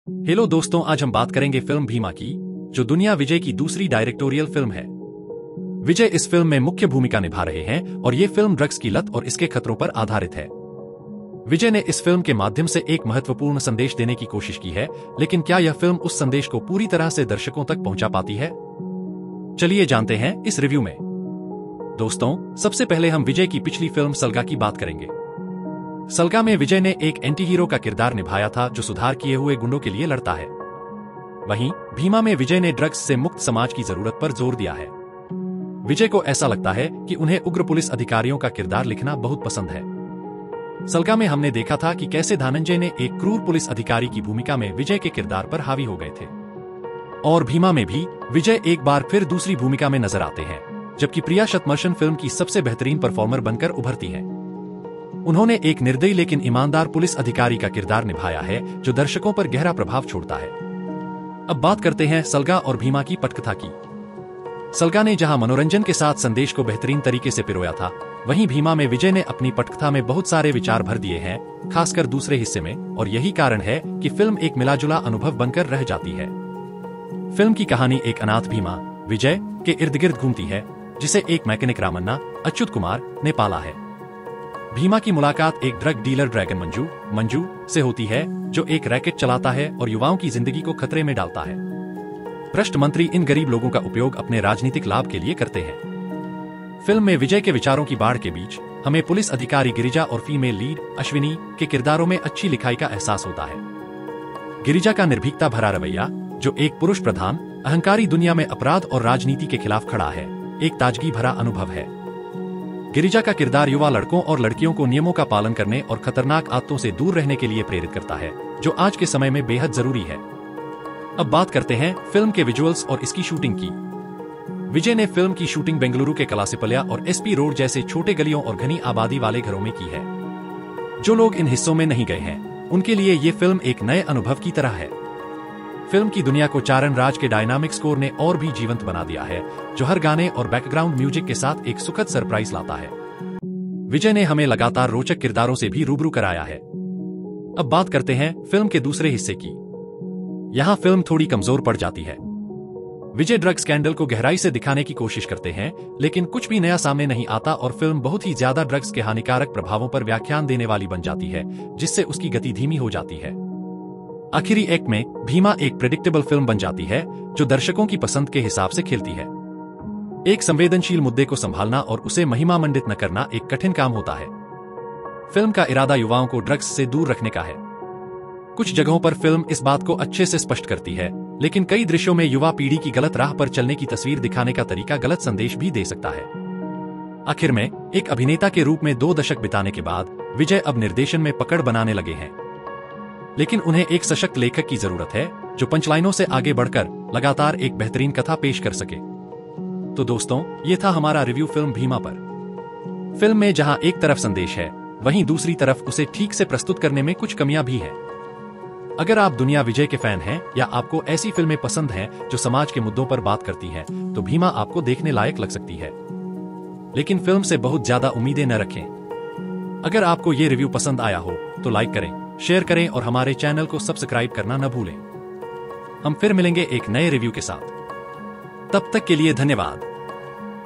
Bheema Movie Review | ड्रग्स के खिलाफ विजय का मिश्रित संघर्ष !
दुनिया विजय की दूसरी डायरेक्टोरियल फिल्म 'भीमा' ड्रग्स की लत और इसके खतरों पर आधारित है। फिल्म में विजय ने एक एंटी-हीरो की भूमिका निभाई है जो ड्रग्स के खिलाफ लड़ाई करता है। प्रिया शतमर्षन ने एक ईमानदार पुलिस अधिकारी के रूप में प्रभावशाली प्रदर्शन किया है। हालांकि, फिल्म की पटकथा में विजय ने विचारों की भरमार कर दी है, जिससे दूसरे हिस्से में फिल्म की गति धीमी हो जाती है। जबकि फिल्म एक महत्वपूर्ण संदेश देने का प्रयास करती है, कुछ दृश्य गलत संदेश भी दे सकते हैं। विजय का निर्देशन बेहतर हो रहा है, लेकिन उन्हें एक मजबूत लेखक की जरूरत है।